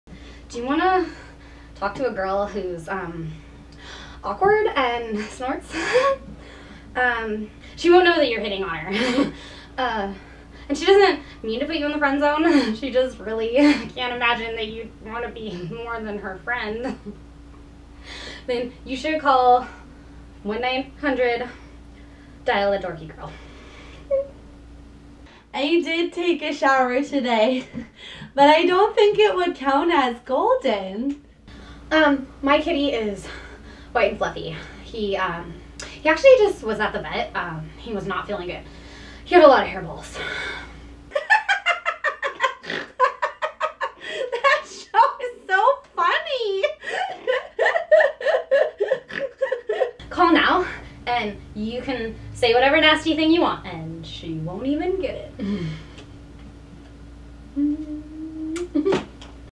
1-900 Dial a Dorky Girl
Commercial for 1-900 hotline to talk to dorky girls - Watch episode 3 of On My Level